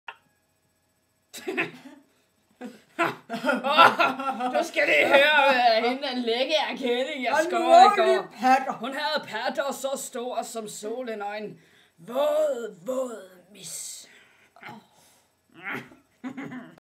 Svend og Bjarne En Våd Våd MIS
Dét er hvad en aften, med eyeliner, og sin roomie kan blive
til når der ikke er andet at tage sig til..